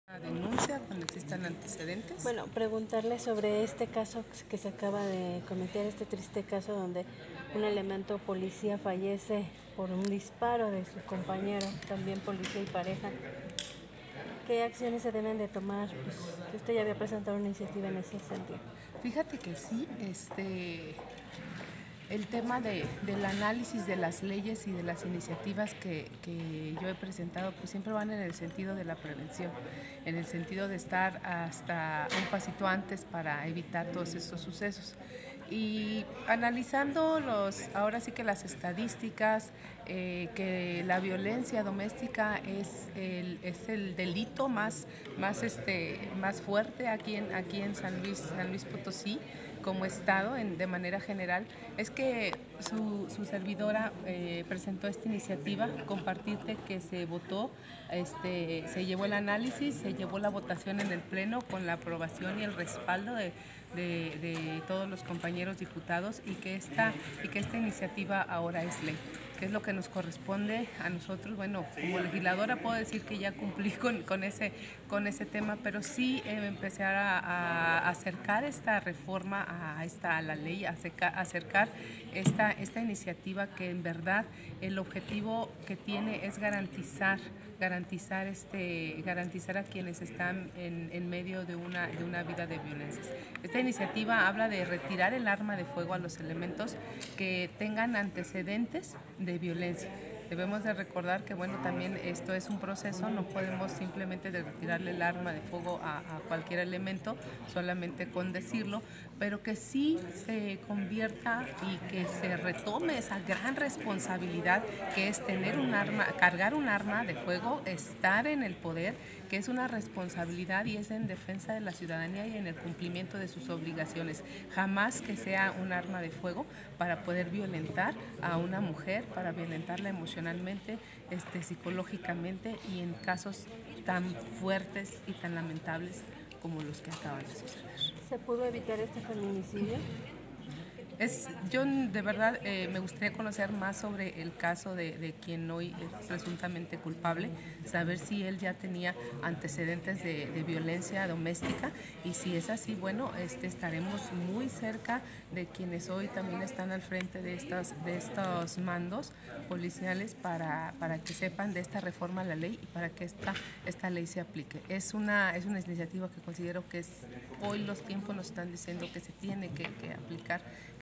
Diputada propone desarmar a los policías agresivos
El Congreso del Estado pide desarmar a las corporaciones policiacas a los elementos de la policía que tienen problemas de agresividad, así lo informo la diputada Liliana Almazán Flores.